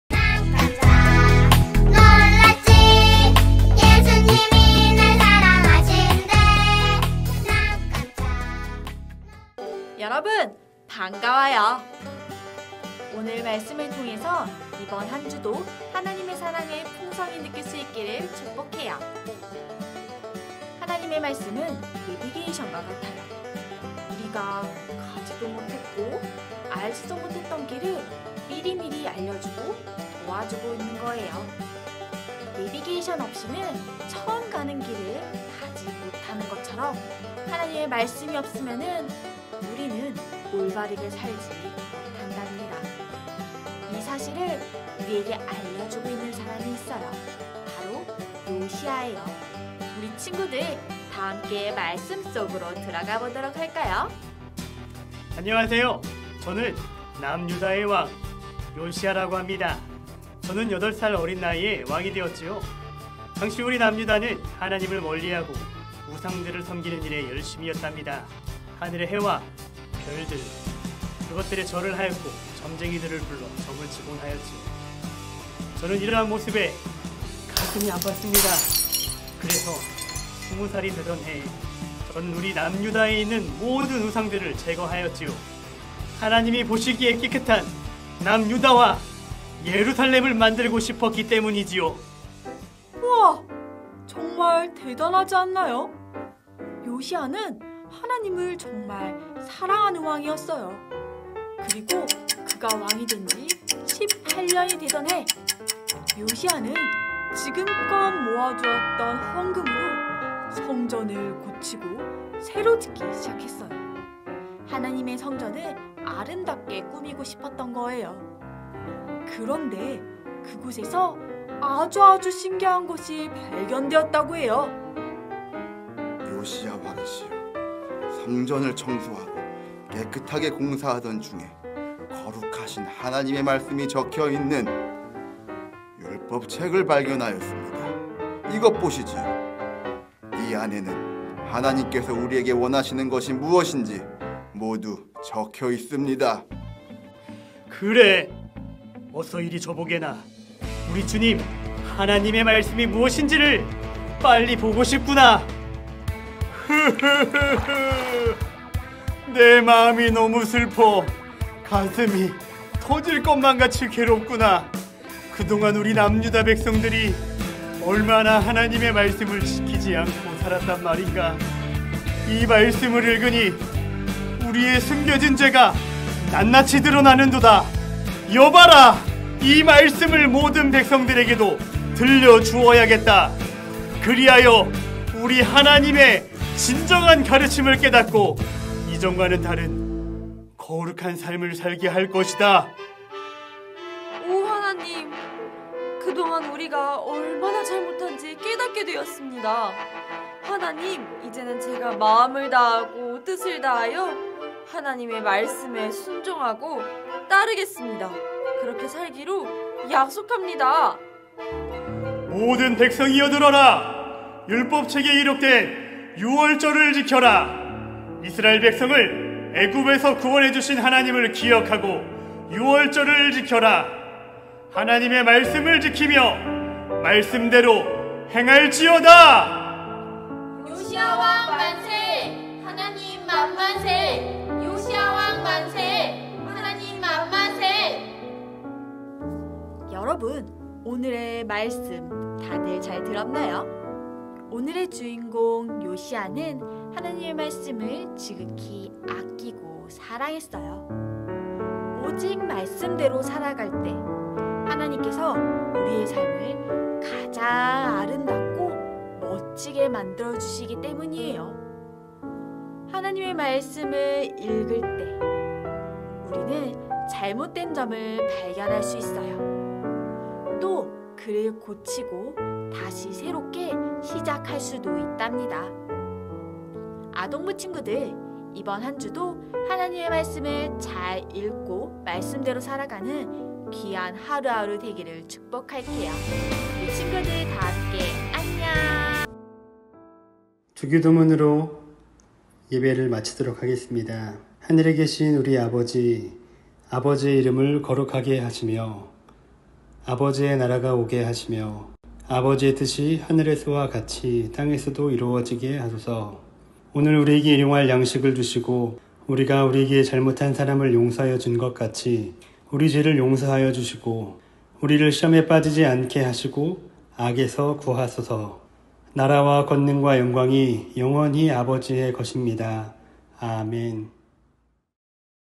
옥포중앙교회 아동부 가정예배영상(2021. 11. 07)
본문 성경말씀 : 열왕기하 22장 3-20절
암송 성경말씀 : 열왕기하 22장 13절